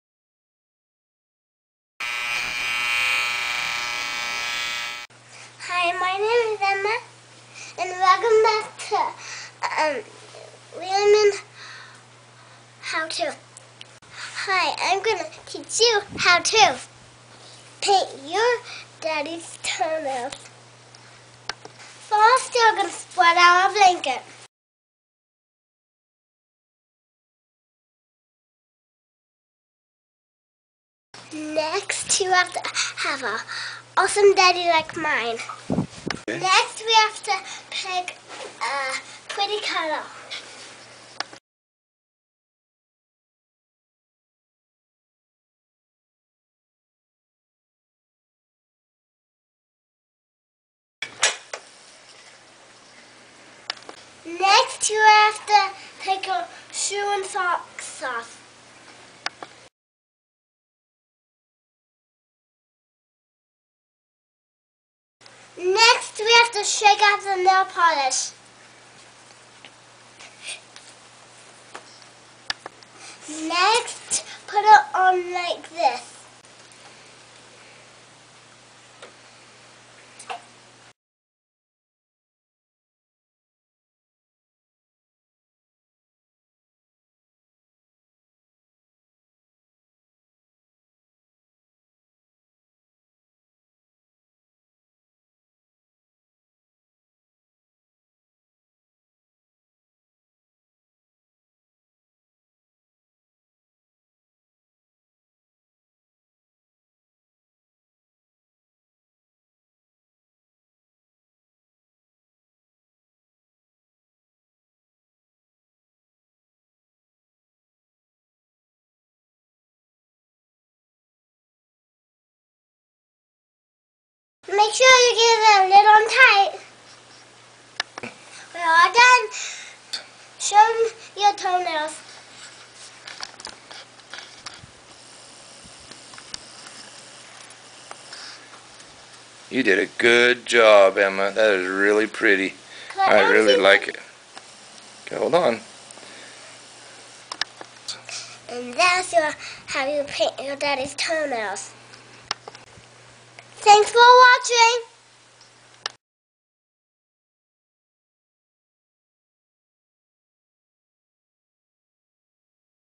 HOW TO Paint your Daddys Toe Nails with Emma (4)
Today Emma will teach you "How To" paint your Daddys toe nails!
Emma is 4 years old and LOVES nail polish!

My RMC "Item of the week at Walgreens" Blog